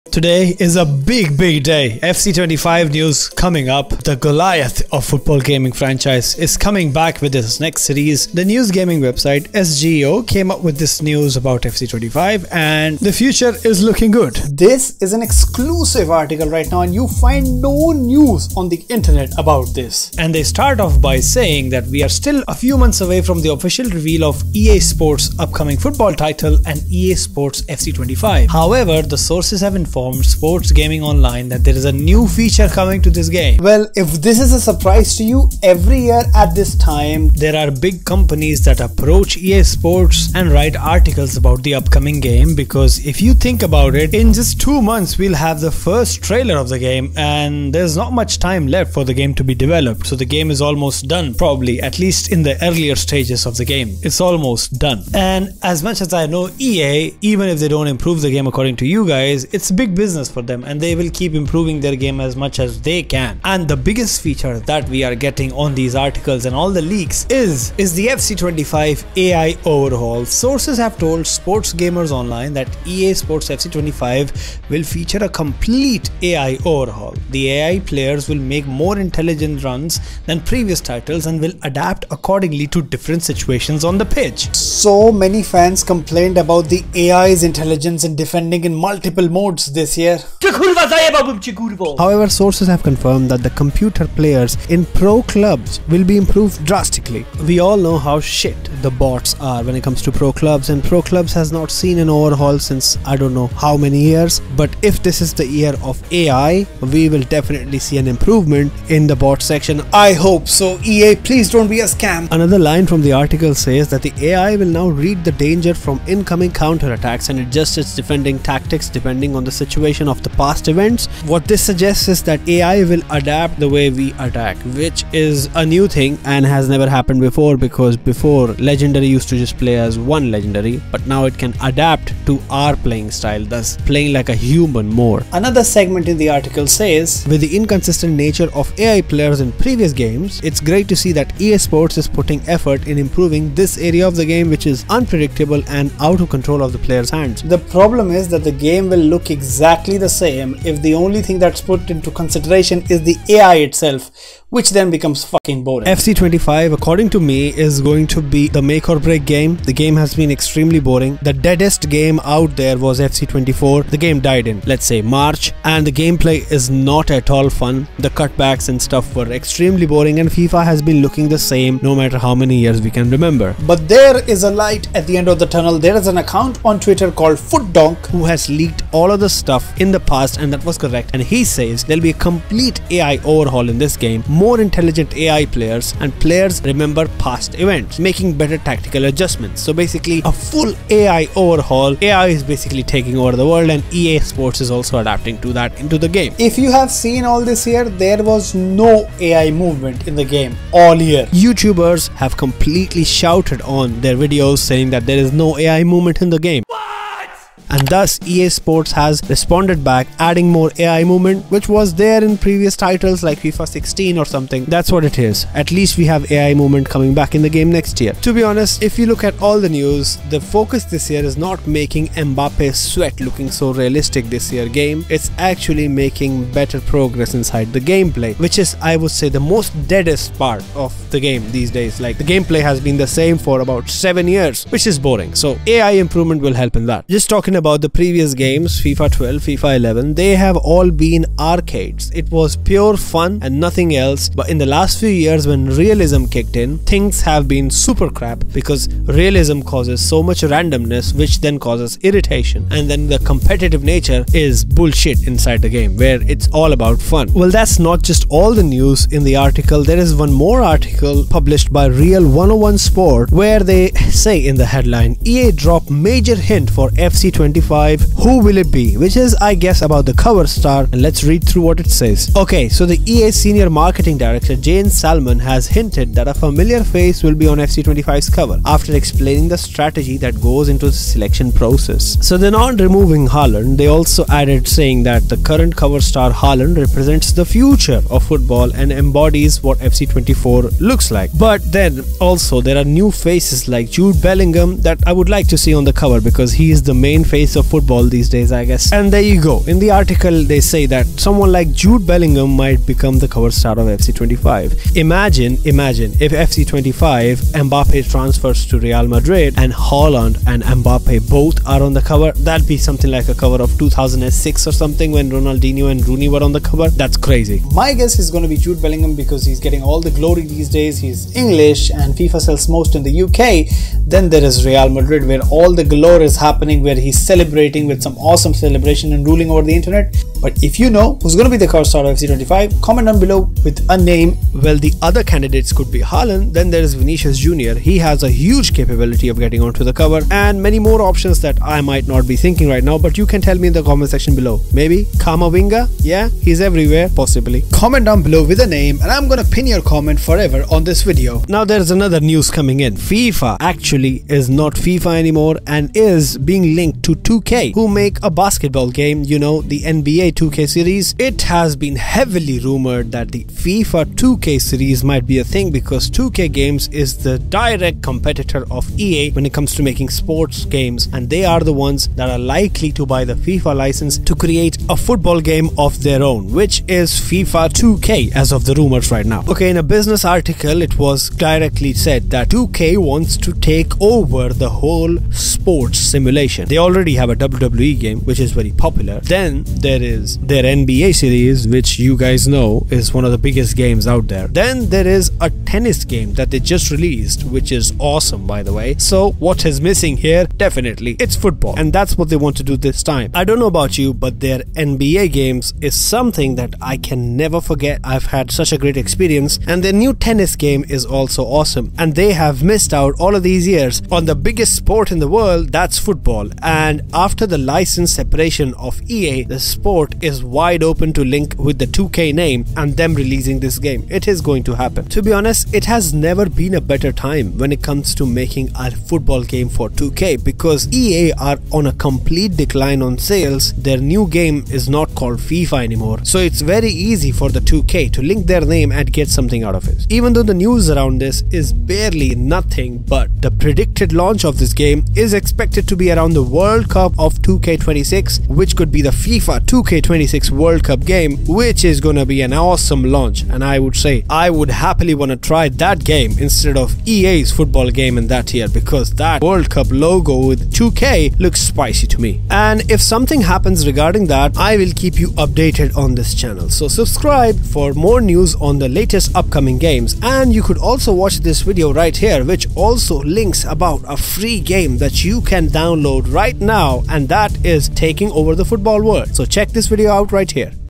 EASPORTS FC 25 is AWESOME!?
Title: EA Sports FC 25: New AI Overhaul & Cover Star Predictions | FIFA 2K Rumors | SGO Exclusive News

Description:
Welcome back to the latest scoop on the highly anticipated EA Sports FC 25! Sports Gamers Online (SGO) just released an exclusive article detailing what we can expect from EA Sports' next big football game. This video dives deep into their revelations and other exciting updates surrounding FC 25.

Highlights:

AI Overhaul Announcement
EA Sports FC 25 will feature a complete AI overhaul, as confirmed by sources at SGO. The AI players will have more intelligent movement, adapting to different in-game situations. This improvement addresses long-standing issues with the AI’s defensive intelligence and inconsistent gameplay.
Cover Star Speculations
Real 101 Sport hints at potential cover stars for FC 25. Senior marketing director Jane Salmon reveals the strategy behind the cover star selection. Could it be Jude Bellingham, Vinicius Jr., Kylian Mbappe, or Erling Haaland? Find out our predictions in the video!
FIFA 2K Series Rumors
There are rumors that the FIFA license may be acquired by 2K Games to create a FIFA 2K series. With 2K's success in NBA and WWE gaming, this could be a massive move to challenge EA Sports in the football simulation market.
Future of Football Gaming
As EA Sports moves away from the FIFA license, the football gaming landscape is evolving. The potential FIFA 2K 26 World Cup game could shake things up. We discuss how this could impact EA Sports and the football gaming community.
Key Features of FC 25:

Full AI overhaul: More intelligent attacking and defending
Tactical adjustments based on past events
Pro Clubs AI improvements
Enhanced gameplay realism
Don't Miss Out: